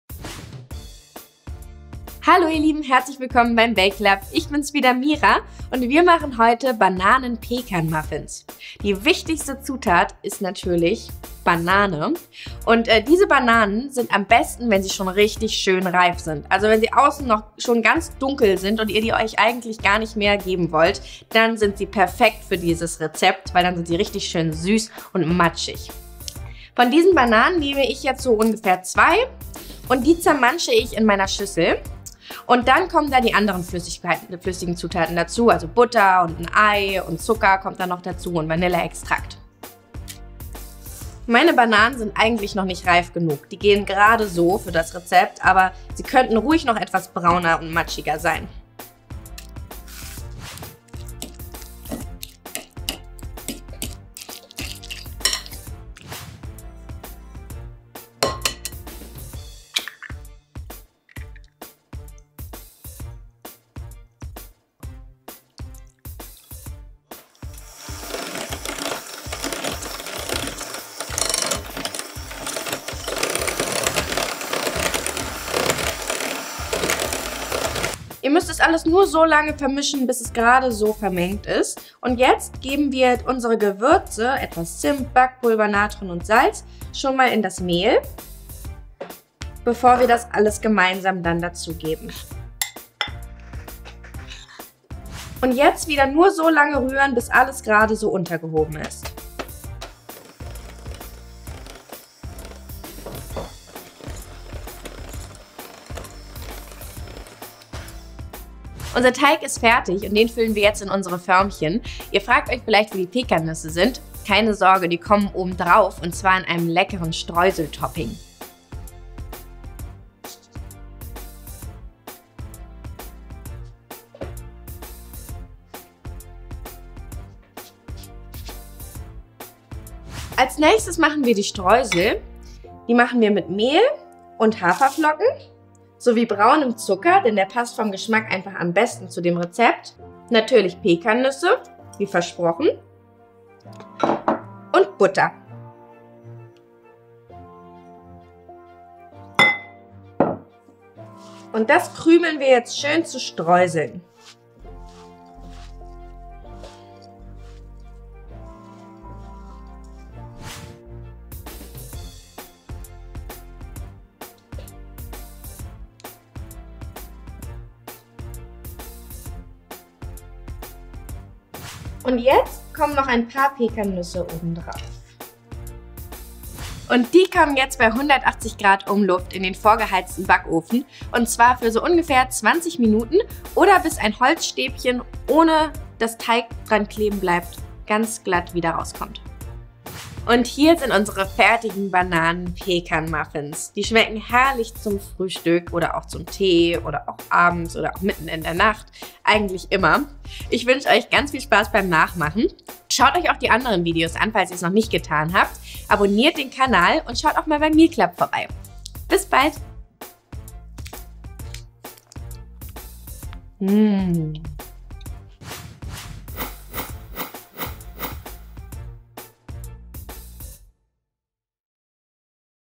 Bananen-Pekan-Muffins | BakeClub
► Hier alle Infos zum Rezept:

Für 12 Bananen-Pekan-Muffins:

Für die Muffins:
- 2-3 Bananen
- 130g brauner Zucker
- 1TL Vanille-Extrakt
- 1 Ei
- 120g Butter
- 200g Mehl
- 1TL Zimt
- 1TL Backpulver
- 1TL Natron
- Prise Salz

Für die Streusel:
- 40g Mehl
- 40g brauner Zucker
- 35g Haferflocken
- 50g Butter
- 60g Pekannüsse

Backzeit: Bei 180°C Umluft für 20min.

BakeClub folgen und keine Videos mehr verpassen!

Viel Spaß mit dem Video und natürlich auch beim Naschen und Genießen!

Bis zum nächsten Mal bei BakeClub!